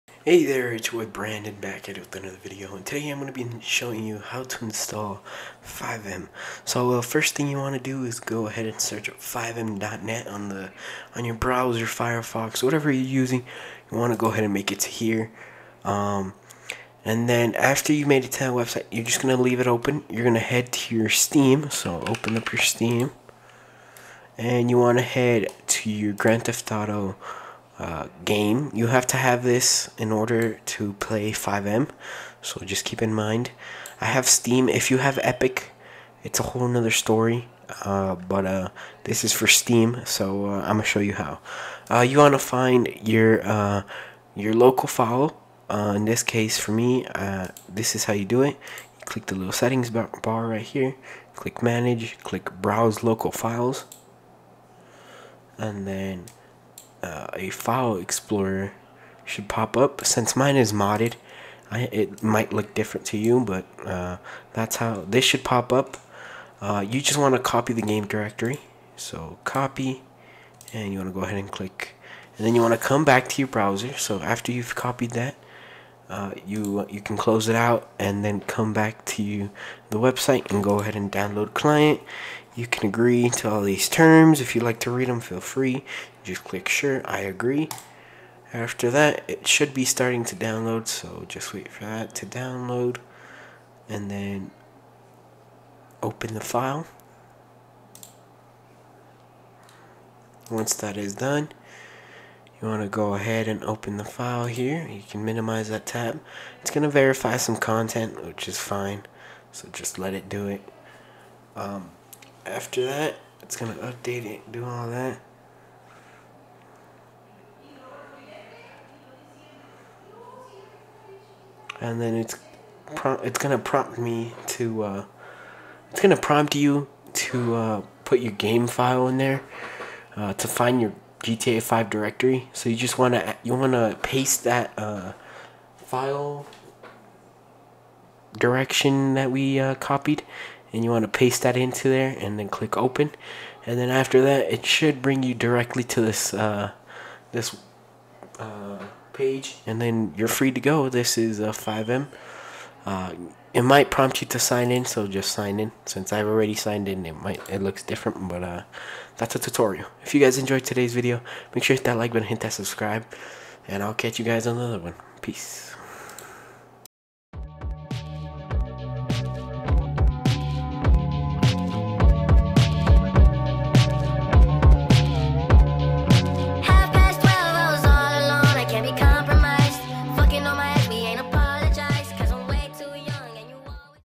How To Install FiveM On PC - Full Tutorial
Learn how to install FiveM on your PC with this step-by-step tutorial. Follow along and start enjoying all the features FiveM has to offer in no time!